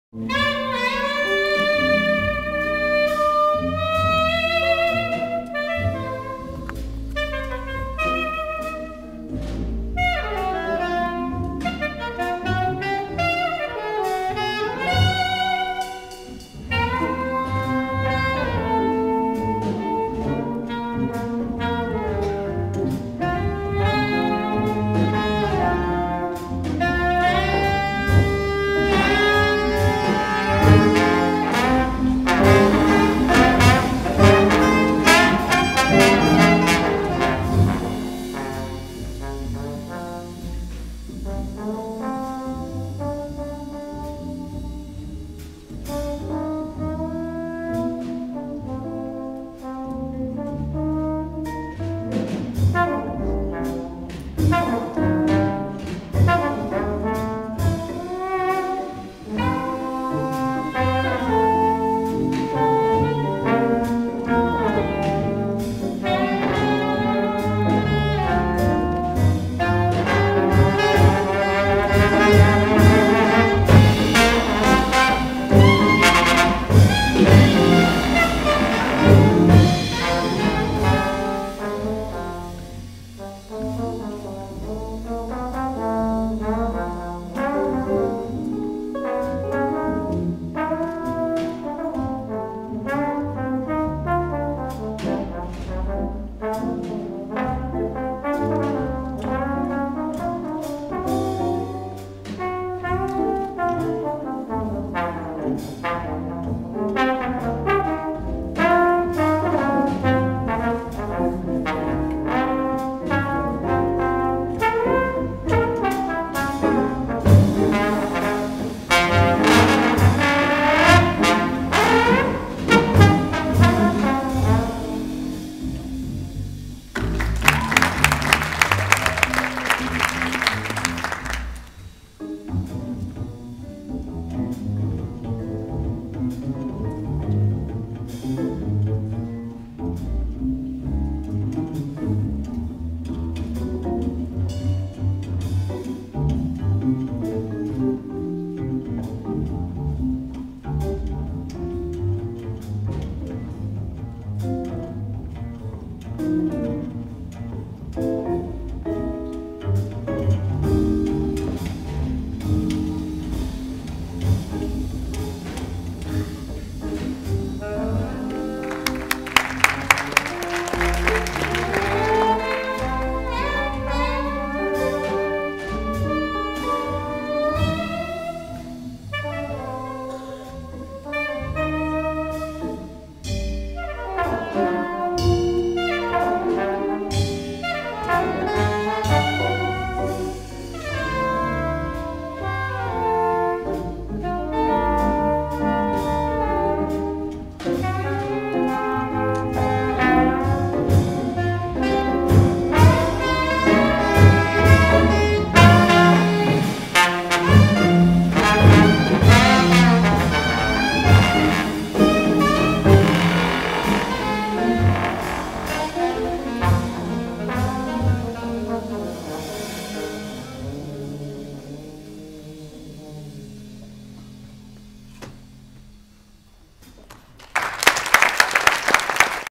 MingusCompetition.m4v
Eastman Youth Jazz Orchestra performing in the National Mingus Comnpetition at the Manhattan School of Music. This is a combo of Alex Cote, Nelson Campbell, Damen Patrick, Eric Stabnow and Ben Tiberio